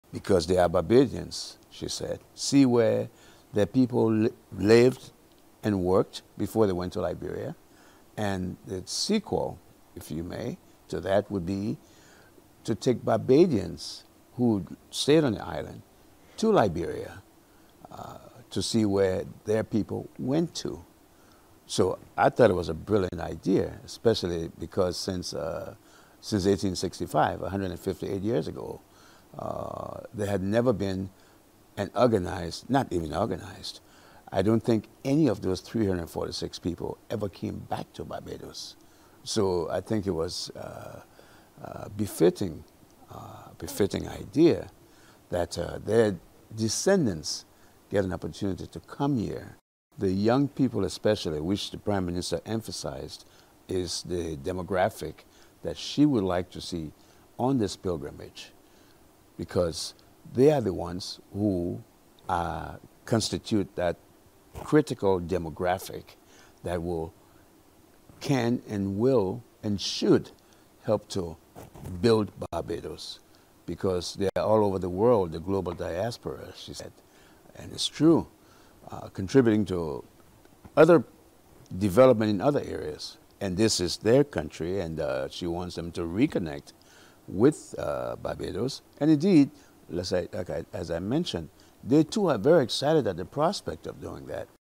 ‘Back to Barbados’ set for 2024
Interest is peaking in West Africa, especially among Liberians for a pilgrimage which will see descendants of Barbadian emigrants to Liberia return to the island.

An event geared toward this, the inaugural “Sankofa Back to Barbados Pilgrimage,” is set to take place from May 6-13, 2024.

Spearheading the pilgrimage is the Africa-Barbados Heritage Initiative - a foundation founded by retired ambassador Lorenzo Llewellyn Witherspoon.

He revealed on CBC TV8's Mornin' Barbados the idea was first proposed by Barbados' Prime Minister, Mia Amor Mottley.

The pilgrimage is expected to attract hundreds of West Africans.

Mr Witherspoon expects positive spin-offs from the Sankofa pilgrimage to Barbados.